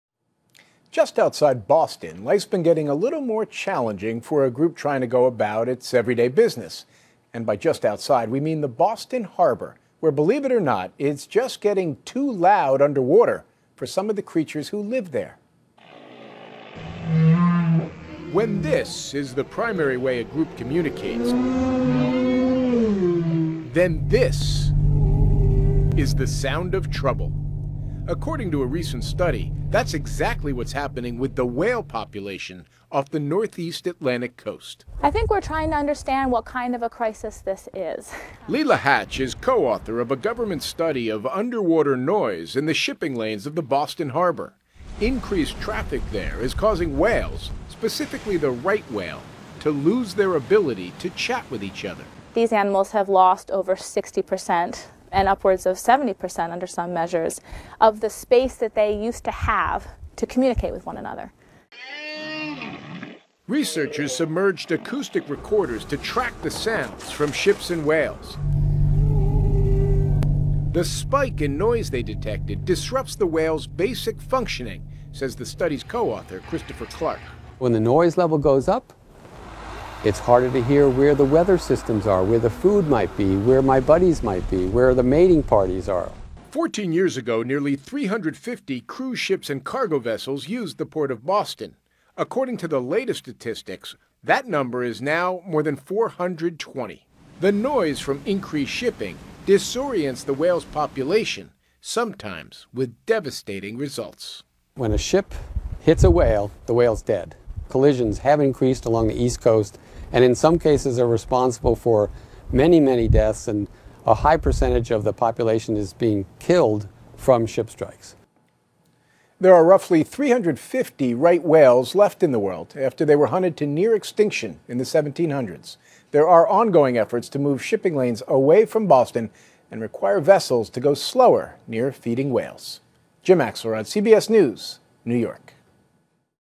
Loud ships disturbing whales
An increasing number of noisy ships traveling through Boston Harbor are disrupting communication between whales. And as Jim Axelrod reports, the underwater acoustic overload can have devastating results.